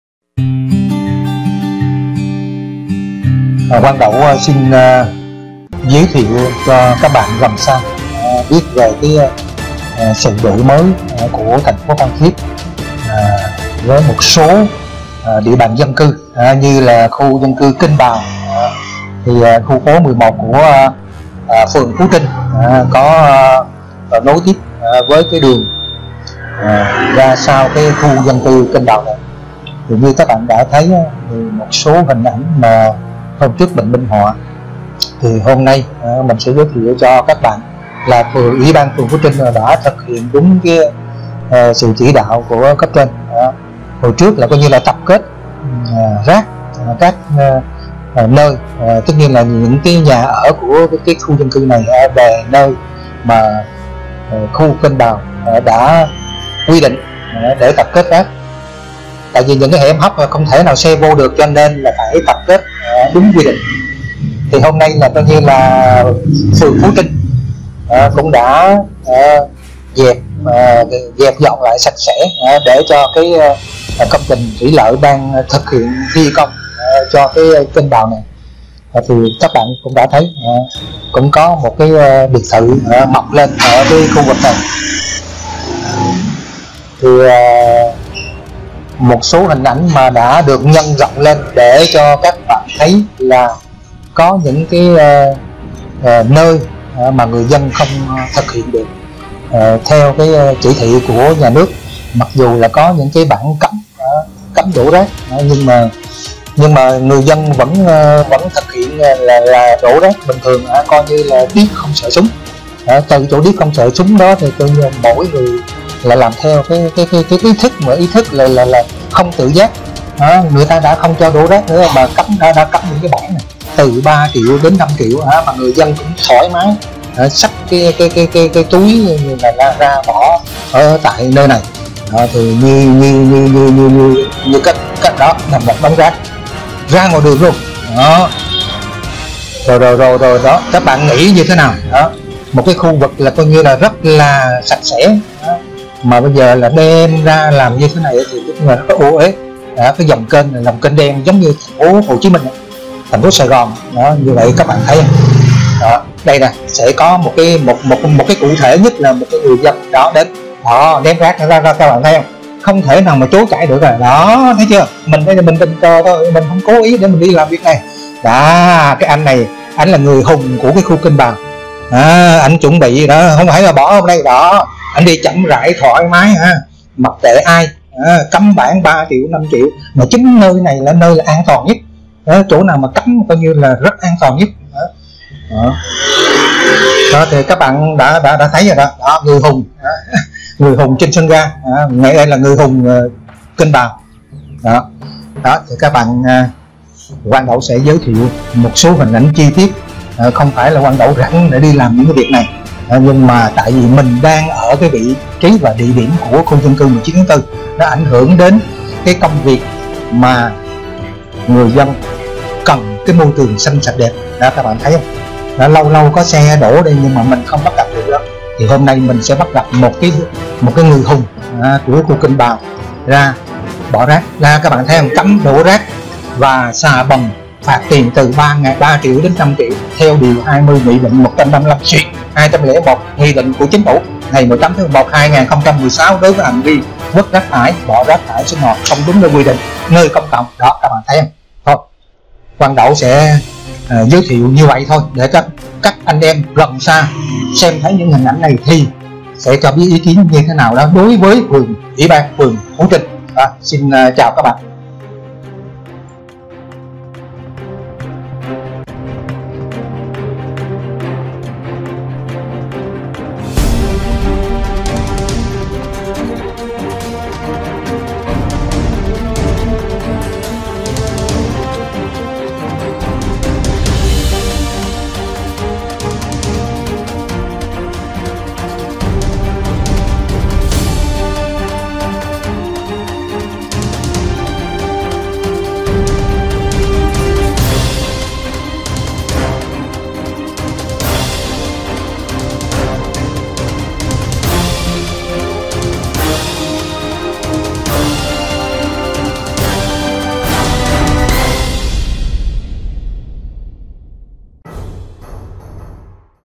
[FULL HD]NGƯỜI HÙNG TRÊN KÊNH BÀU PHAN THIẾT
PHẢN ÁNH CUỘC SỐNG THỰC TẾ
HIỆN HỮU BÌNH THƯỜNG MỚI
NGƯỜI HÙNG TRÊN KÊNH BÀU PHAN THIẾT
GÓC NHÌN VỀ MỘT THÀNH PHỐ
TƯƠNG LAI XANH SẠCH ĐẸP
TỪ RÁC RƯỞI SẶC MÙI NƯỚC CỐNG
PROMOVIES VIDEOKAR NS QUANG DAU